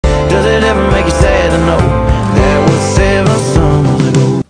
When you have all 5 of your babies in 7 years 🤪🤗❤️
We had our first baby in the fall of 2015 and our fifth baby in the fall of 2022!

To think just a few summers ago it was JUST Tim and I hurts my brain a bit 😝

Does it make me sad like the song asks? Absolutely not! In fact I don't even remember much of my life before I became a mom.

Some may think that's sad. Some may say have lost my identity in motherhood, but I say it's been quite the opposite for me. I have found myself through this journey.

Do not get me wrong, I had a hard time grieving my old self after my first and even after my second. But it's all part of the process. By becoming mothers we don't lose who we are. Rather, it allows us to shed the parts of ourselves that no longer serve us.

What did summer look like for you 7 summer ago??